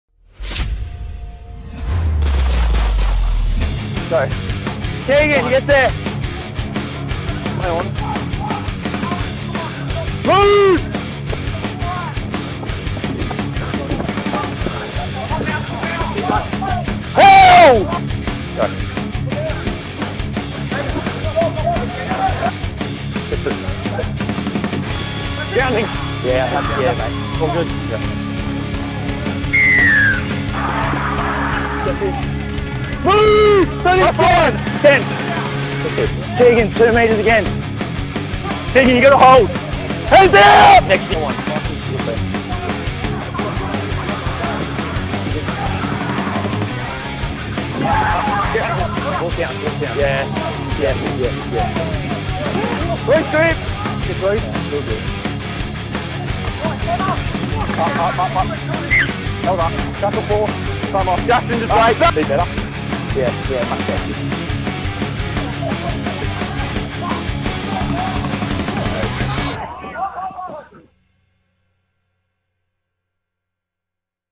Cyril Connell Cup Weapon: Round 4 - Shaun Elara
Scoring tries and saving tries, Western Clydesdales lock Shaun Elara helped keep his side at the top of the Cyril Connell Cup ladder with a strong performance in Round 4.